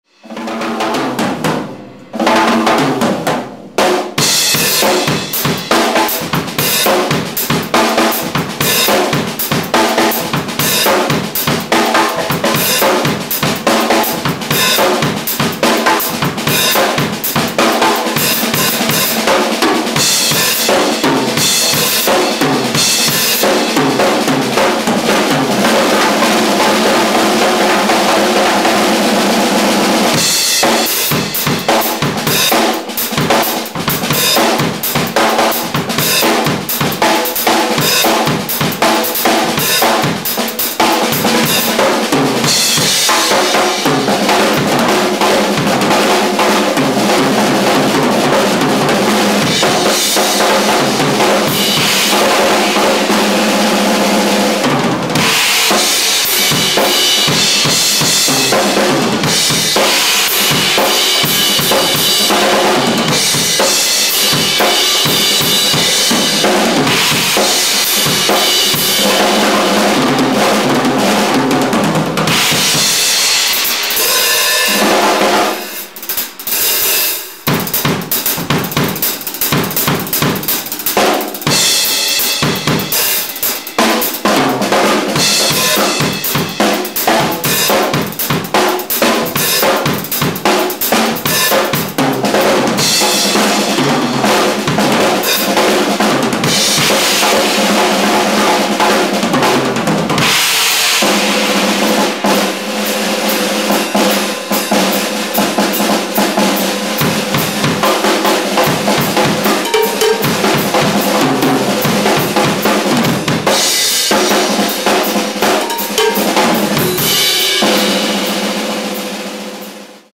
Drumming on my Ludwig White Marine Pearl kit
That's what drummers do without any other musicians around.
All Ludwig classic maple drums in WMP 8"x12", 14"x14", 16"x16", 16"x24" the LM402 6.5"x14" metal snare and a 5"x14" birch snare in silver rap.
Cymbals are all Zildjian, 14" New Beat Hi-hat's, 16" & 17" K-crash, 20" K-Custom Dark Ride & a 22" Oriental China Trash.
hears are Remo, coated Emperors on top & Clear Ambassador on bottom for Toms. Coated Ambassador CS on top of Snare, Ambassador Hazy on resonant side. Coated Powestroke3 on kick.